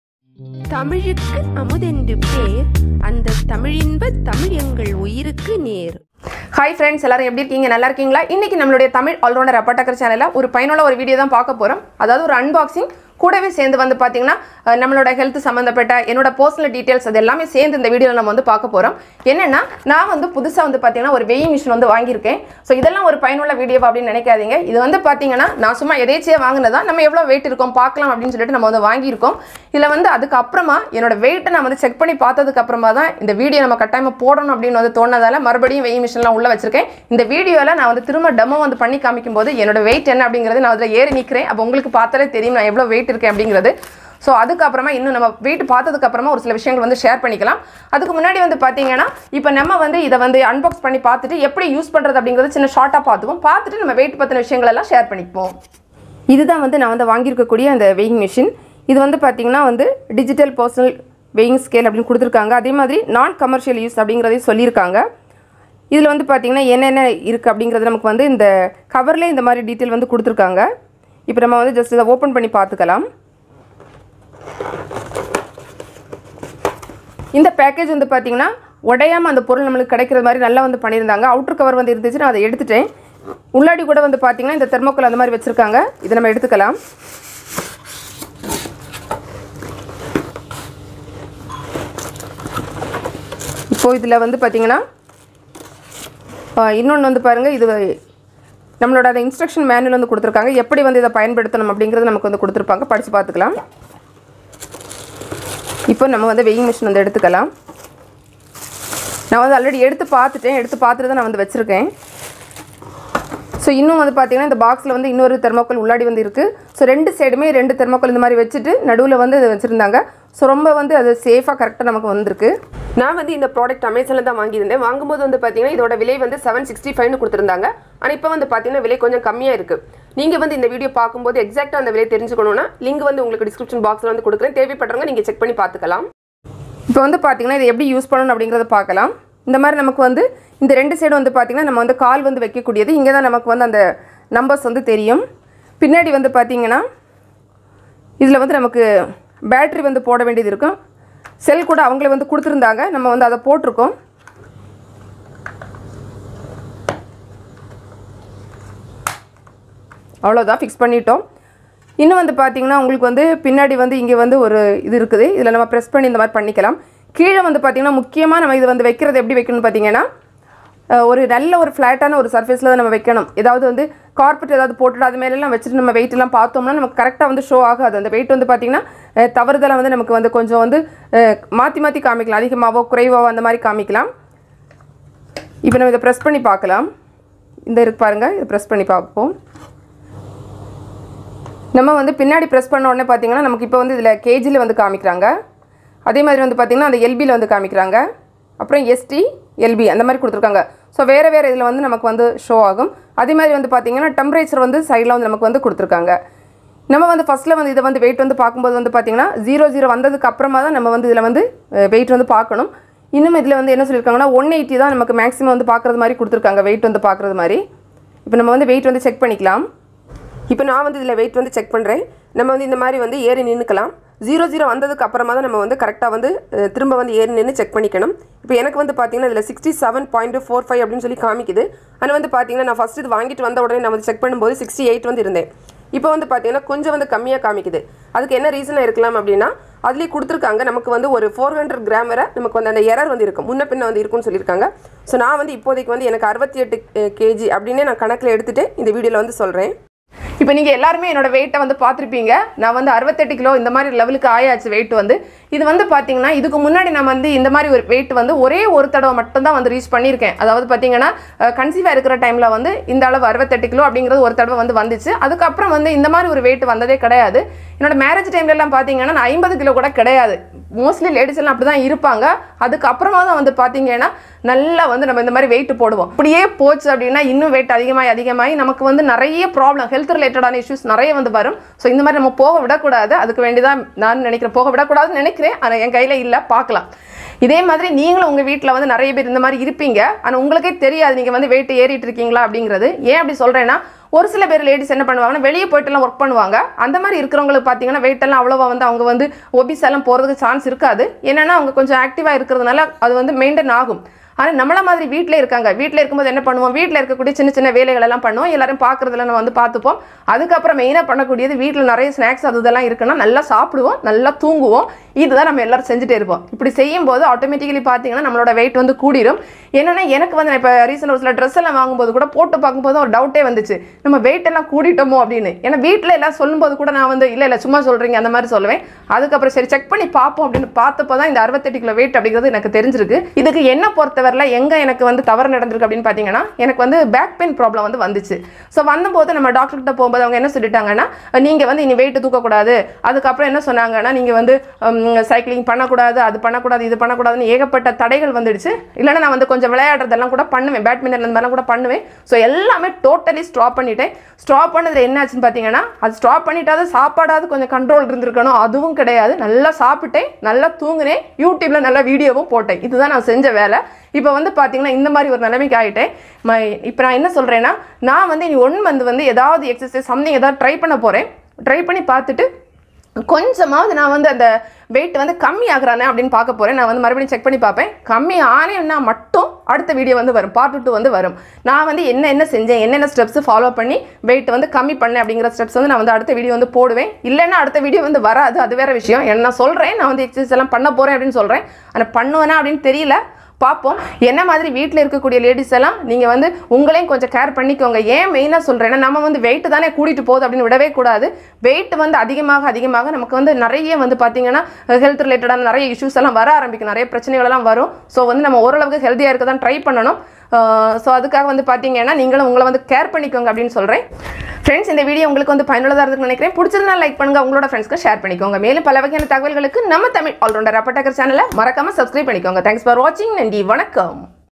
weighing machine amazon / My body weight revealed/why my weight increased / உடல் எடை
Amazon body weighing machine unboxing and my body weight revealed

Digital personal weighing scale how to use

TARA youtube Channel